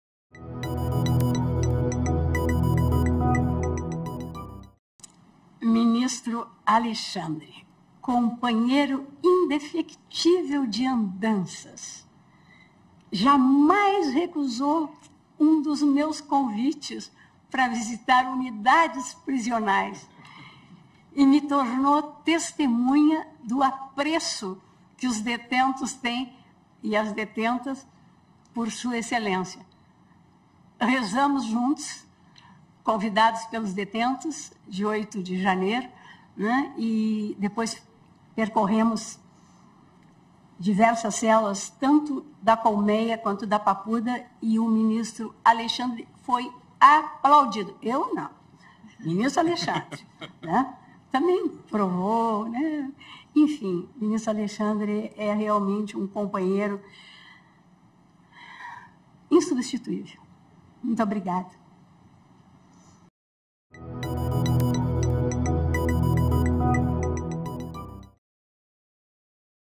Weber diz que Moraes rezou com presos do 8 de Janeiro e foi aplaudido
A ministra Rosa Weber, em discurso no STF (Supremo Tribunal Federal) na 4ª feira (27.set.2023), contou que, durante uma visita ao complexo penitenciário onde estavam encarcerados os presos pelo 8 de Janeiro, ela e o ministro Alexandre de Moraes rezaram com os detentos.

“Rezamos juntos, convidados pelos detentos de 8 de janeiro, e depois percorremos diversas celas, tanto da Colmeia quanto da Papuda, e o ministro Alexandre foi aplaudido”, falou a ministra.